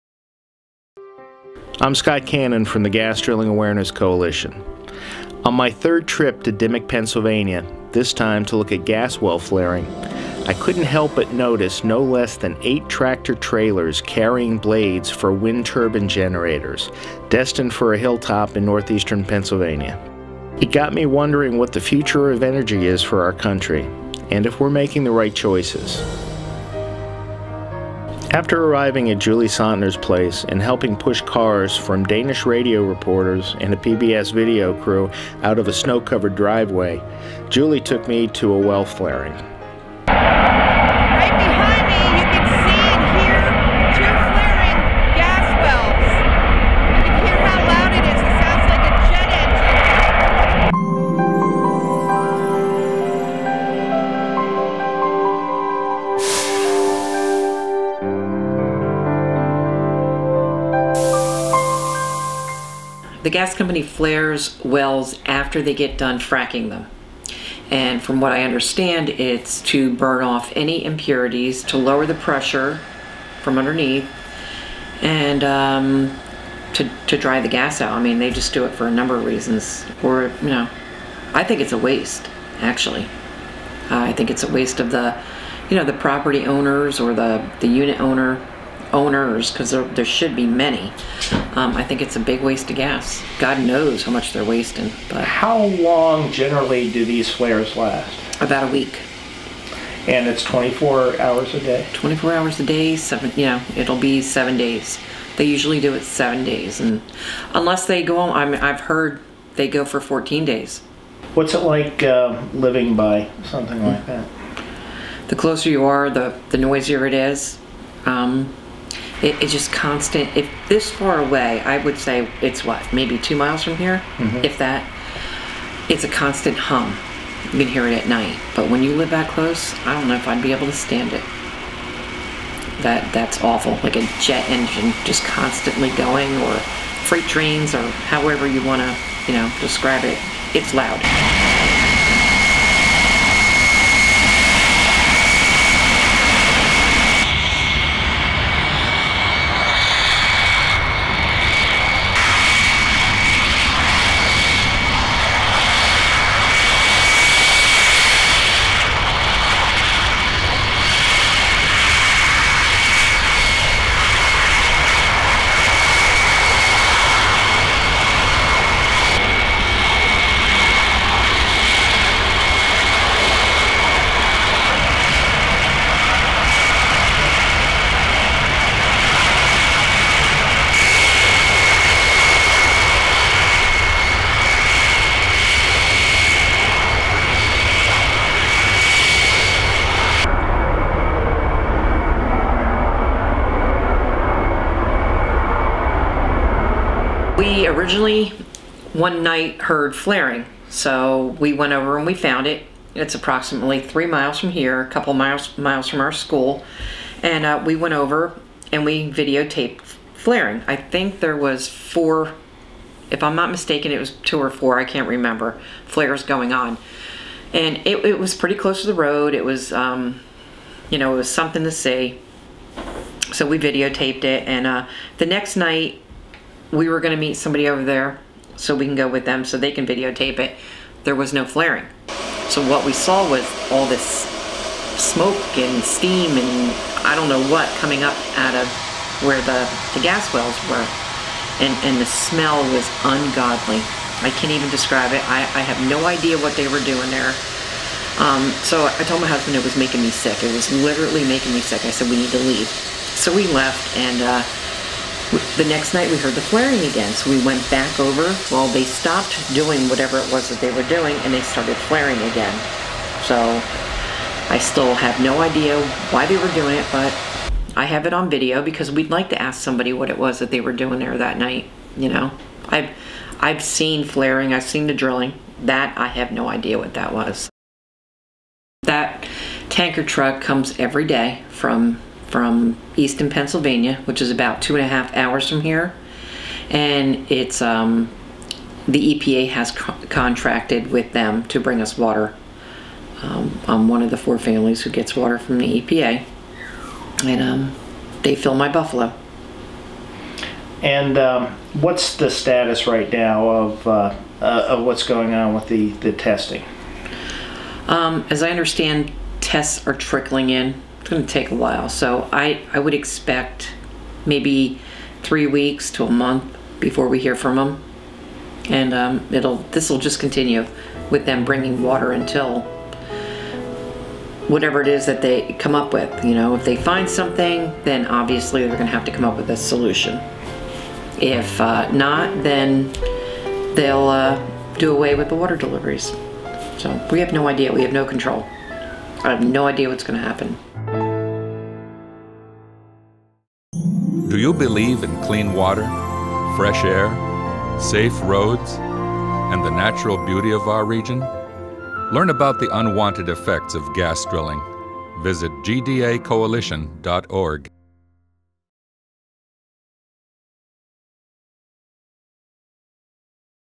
Gas Well Flaring - Marcellus Shale Reality Tour Part 4 - Fracking
A video by Scott Cannon
Julie Sautner takes us to see a gas well flaring and talks about their current situation with water deliveries from the EPA. Gas Drilling Awareness Coalition. Some YouTube clips used by permission from YouTubers veraduerga and hammerfly. Recorded on February 17, 2012 The entire list of Pennsylvania water contaminations from fracking.